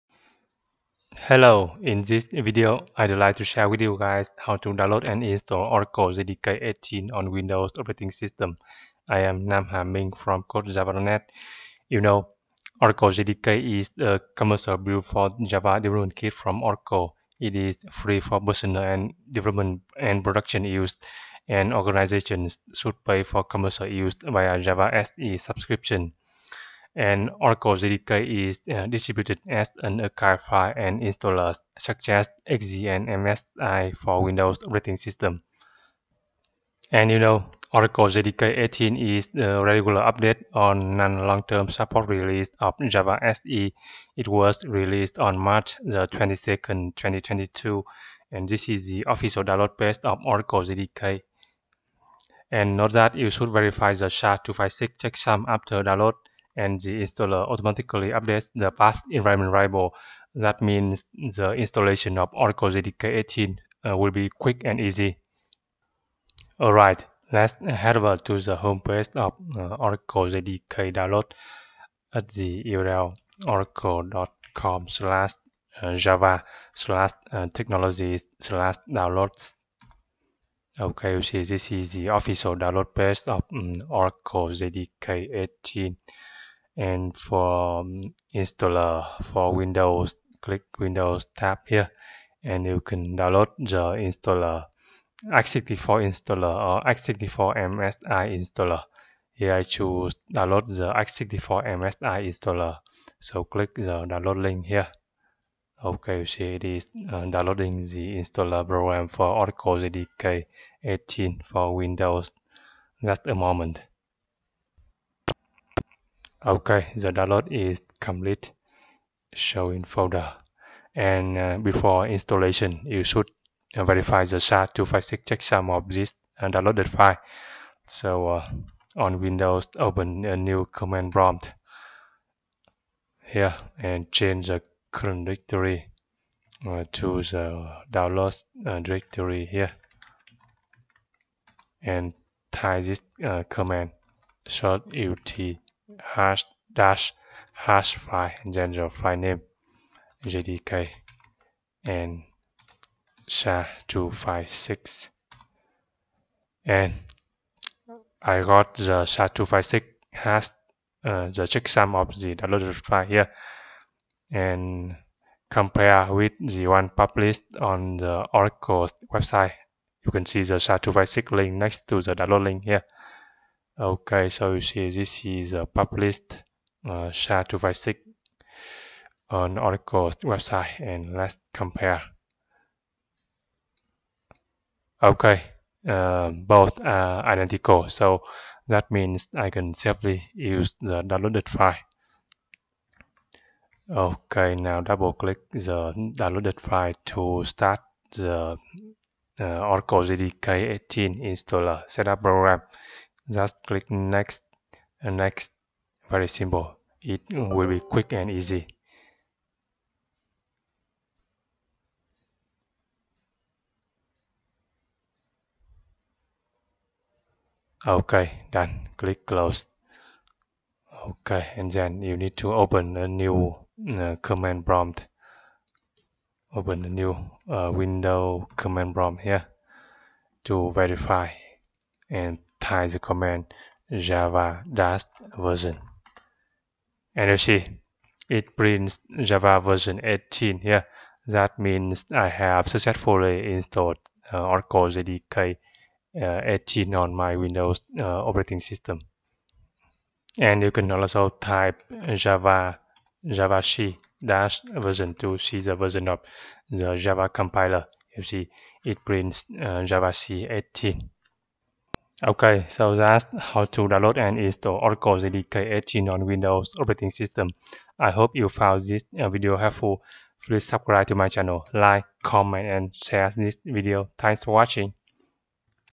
Download and install Oracle JDK 18 on Windows
JDK 18 is a feature update of Java SE Development Kit (Non-LTS). And Oracle JDK is a commercial distribution of JDK, which is free to use in development and production.

In this video, I'll guide you how to download and install Oracle JDK 18 on Windows operating system. Download JDK 18 from the official source, verify checksum and install it.